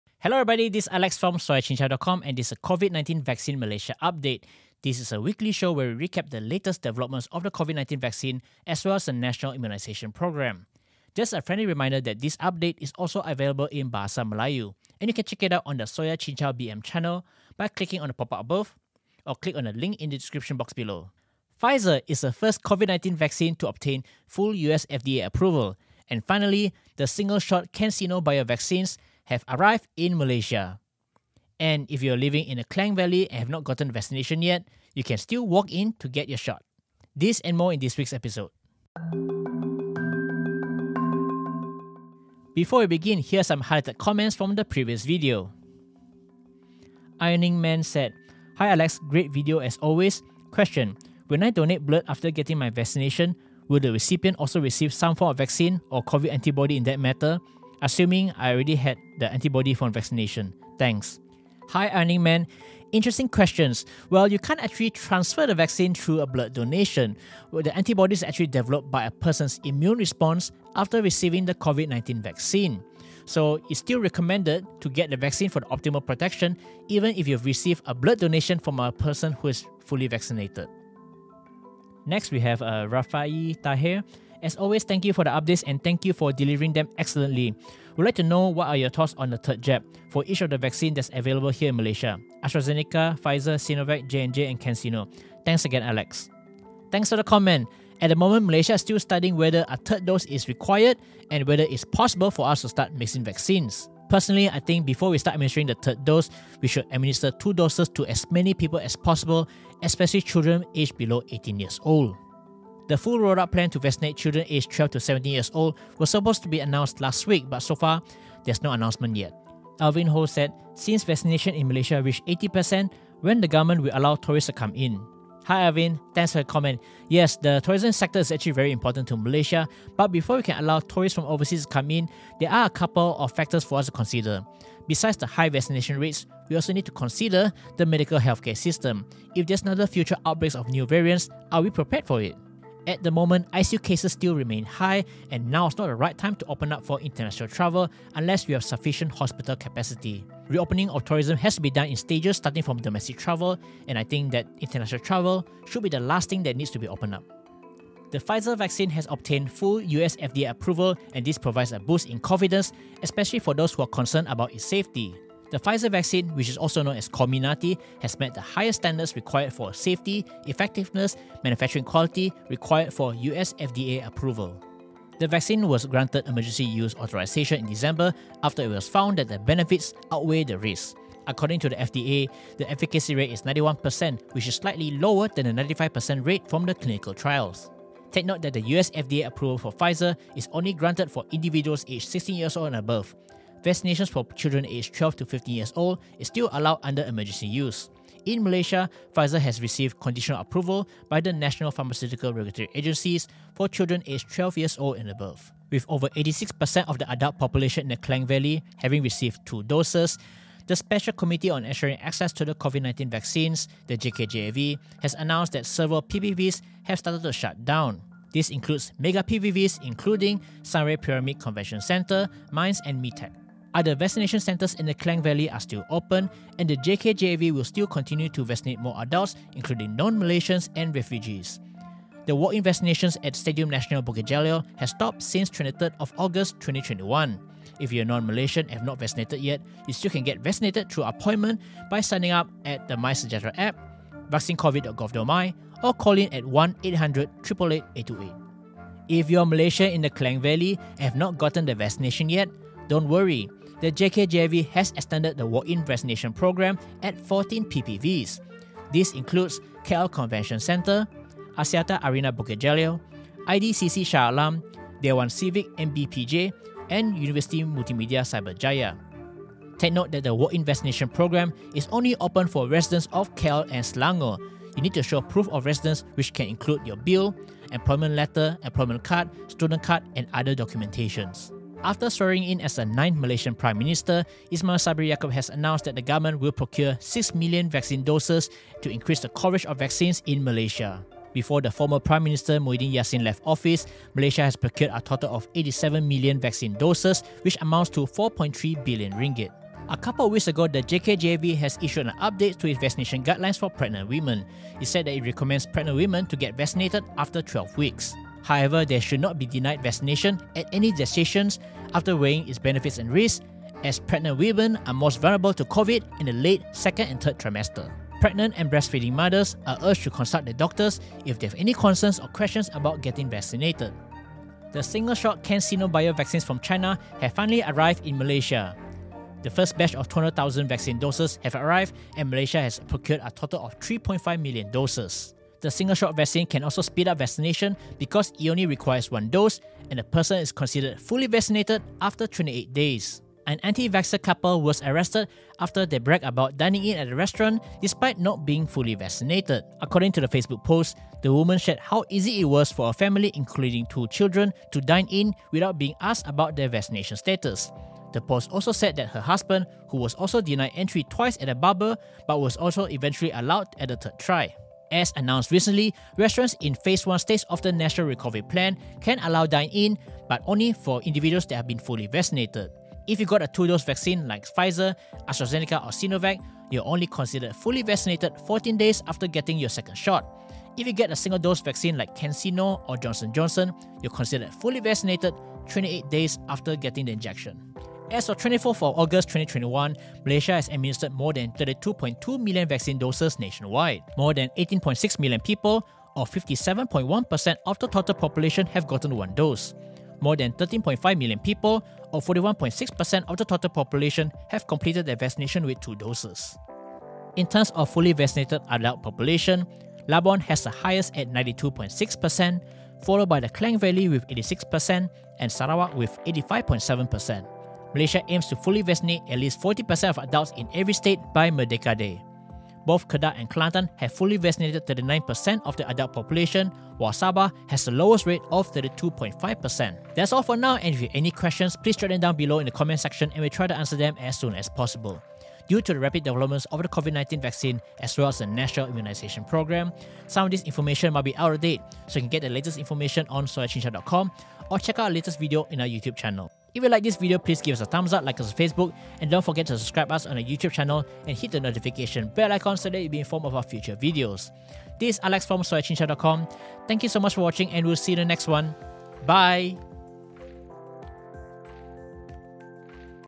COVID-19 Vaccine Malaysia Update #24: Pfizer gets full US FDA approval & CanSino vaccine has arrived
This is the COVID-19 Vaccine Malaysia Update series where we will recap the latest developments of the national immunisation program in Malaysia. This week, we will talk about Pfizer COVID-19 vaccine getting full US FDA approval and the single-shot CanSino bio vaccines have arrived in Malaysia.

You can watch the vaccine update in BM on our SoyaCincau Bahasa Melayu channel:

0:00​​​ - Intro
0:42 - Comments from the previous video
2:37 - Pfizer vaccine gets Full US FDA Approval
3:25 - Klang Valley PPVs are shutting down in stages
4:09 - Walk-in vaccination still available for Malaysians in the KL and Selangor
4:41 - Malaysia procures 6 more million vaccine doses
5:00 - Vaccinations allowed at any period during pregnancy
5:24 - CanSino Bio vaccines have arrived
5:43 - Antivax couple arrested for violating SOP
6:28 - Vaccination Status